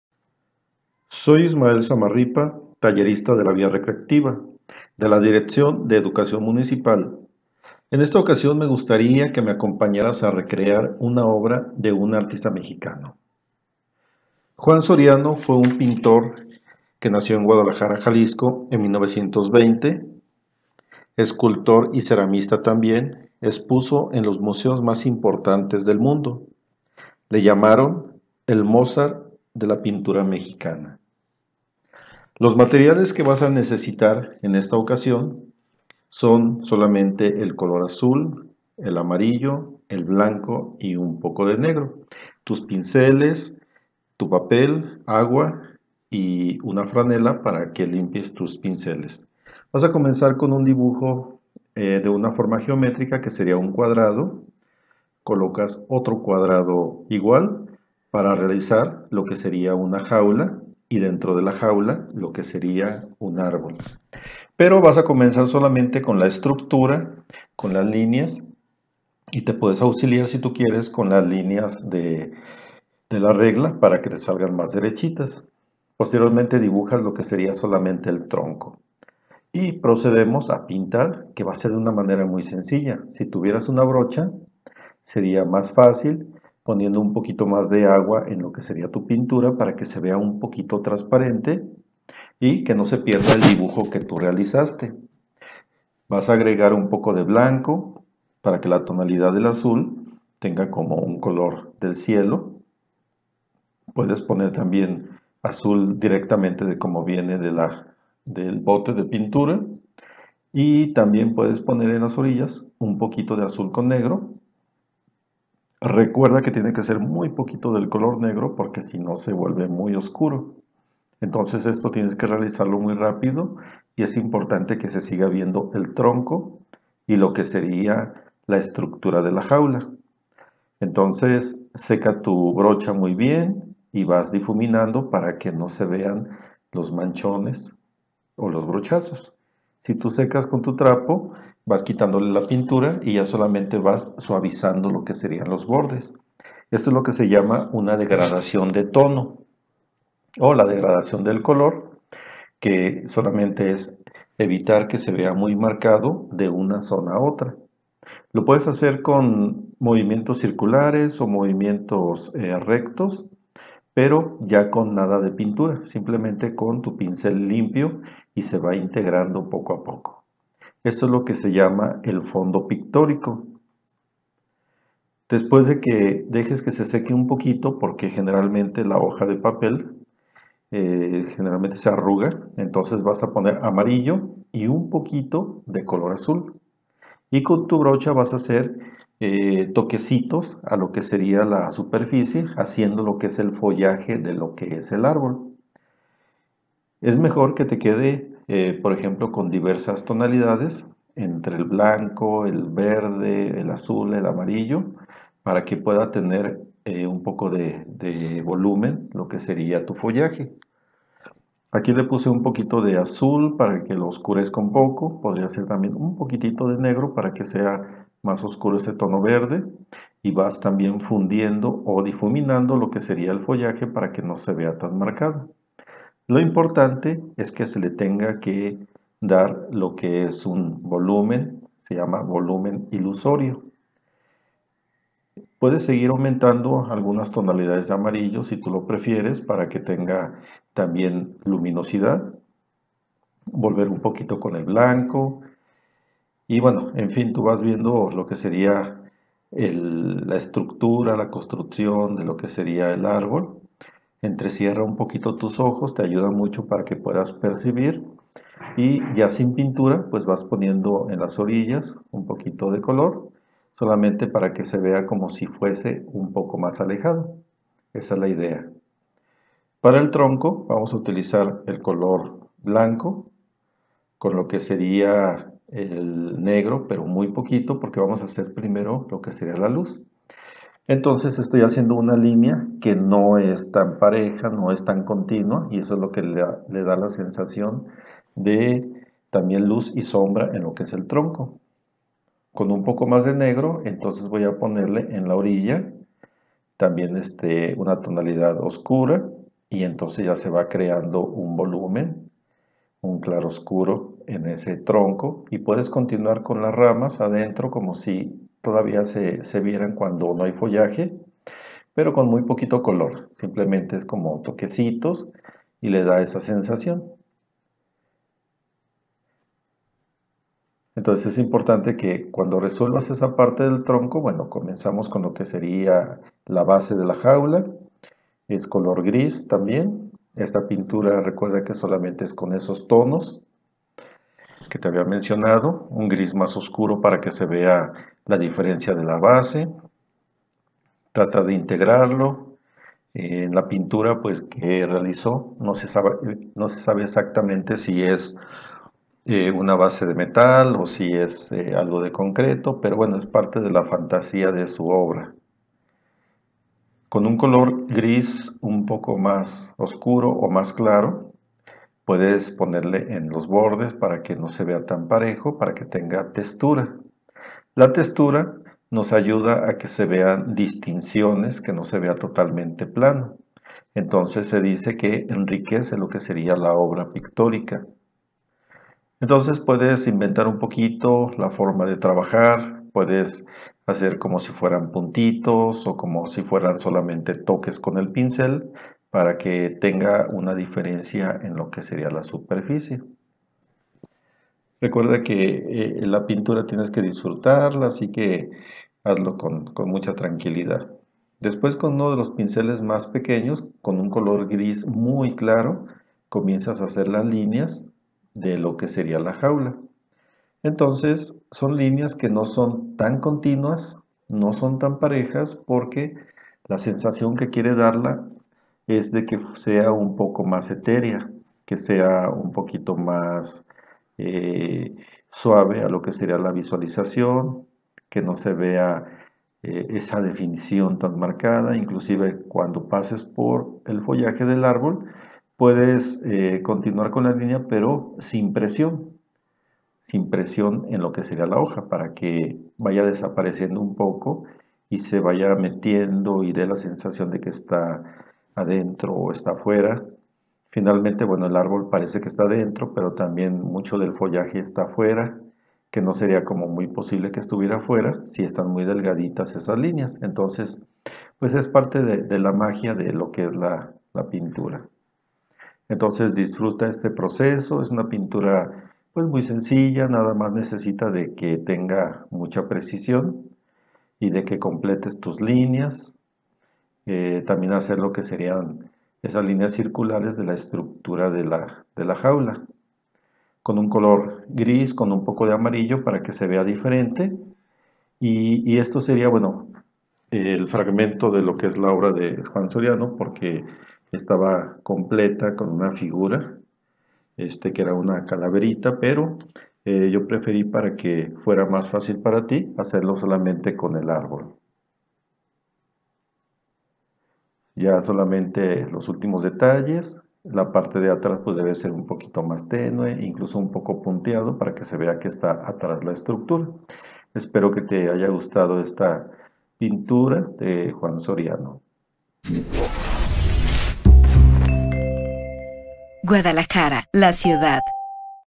Obra de Juan Soriano - Gobierno de Guadalajara
¡Pinta con nosotros una obra de Juan Soriano!

Aprende en nuestro taller de educación a recrear, en compañía de tus peques, una de las obras más conocidas de este multifacético y célebre artista plástico tapatío.

Entérate de todas las actividades que se realizan para construir la ciudad que queremos